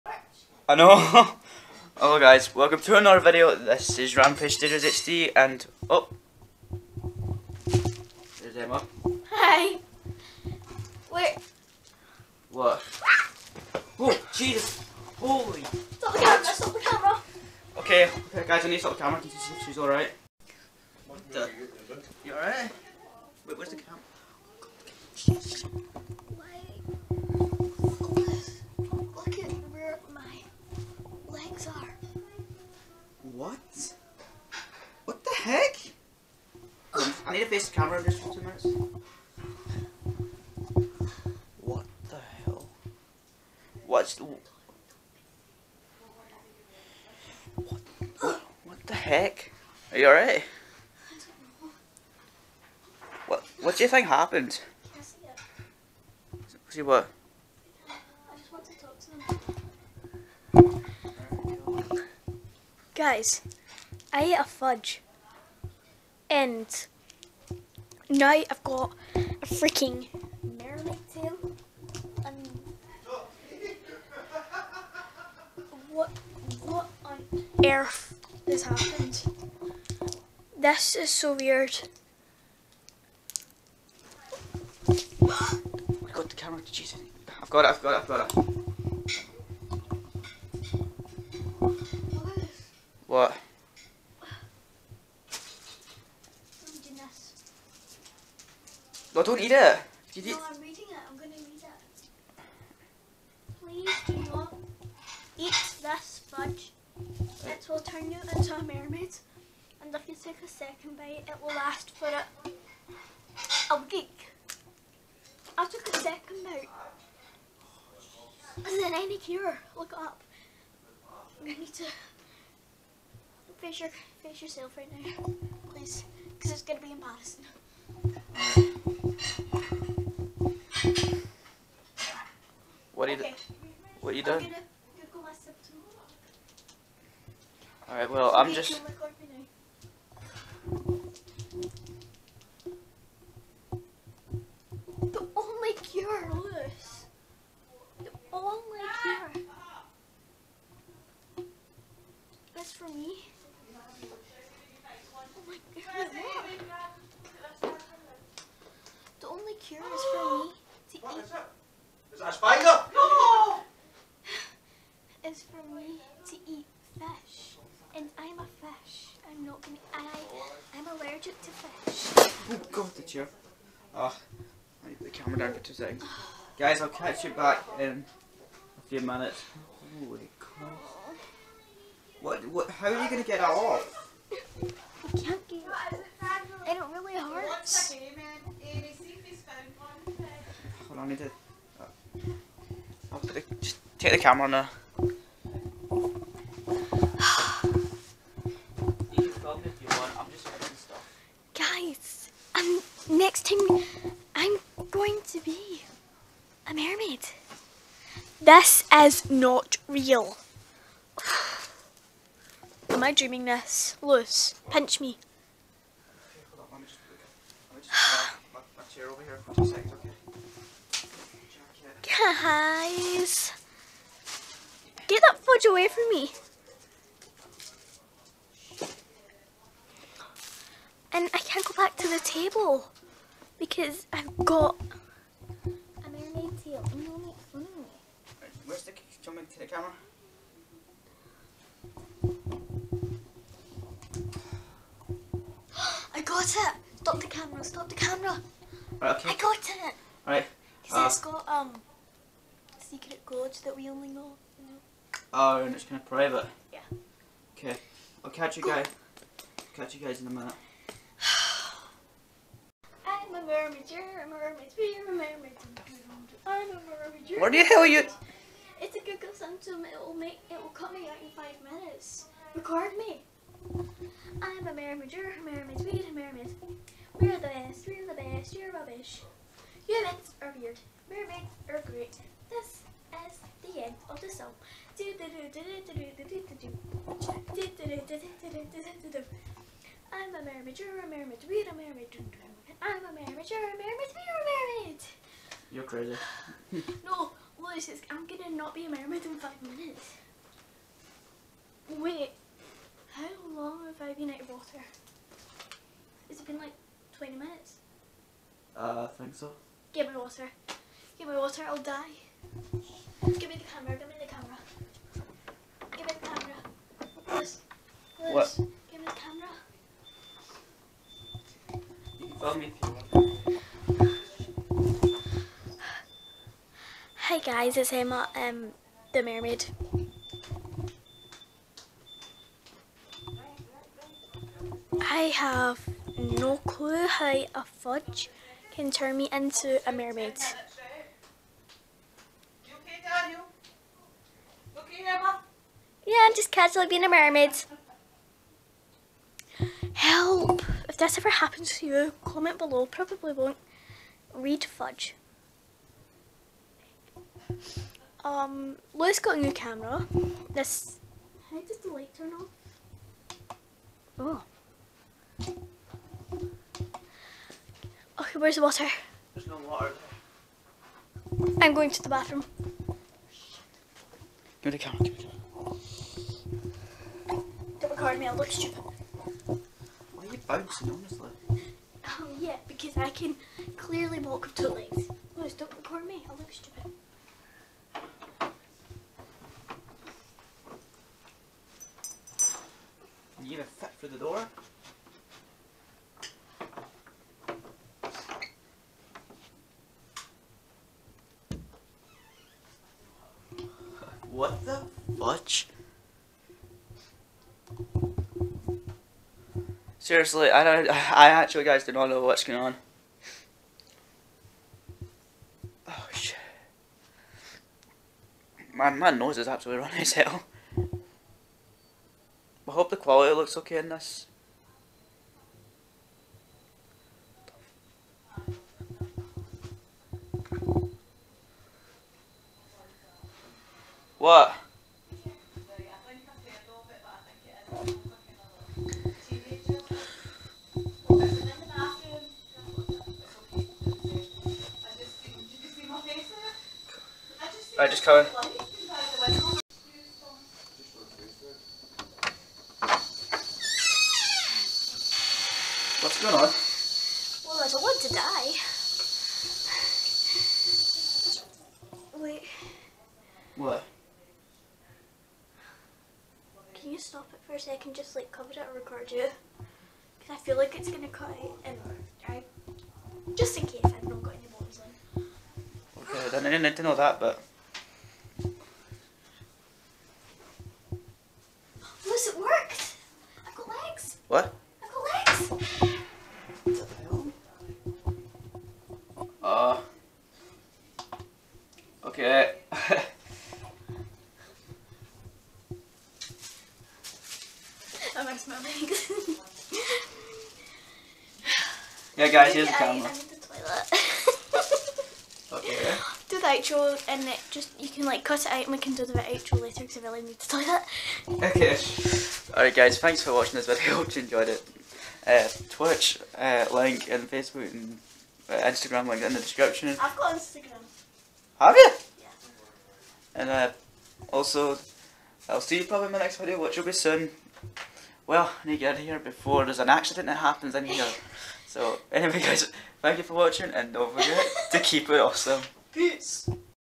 EMMA BECOME'S A MERMAID!
Hey Guys so I was talking to Emma about some awesome stuff and Emma Ate A Bar of ponies Fudge! And became a Mermaid! And don't forget to keep it awesome! I love talking to you guys!. ___________SUBSCRIBE FOR MORE! CONTENT!

__________ below!
Software used
HD Video Camera!
Go Pro HD camera!
iPhone 5 camera
Editing Software!
Easy movie maker!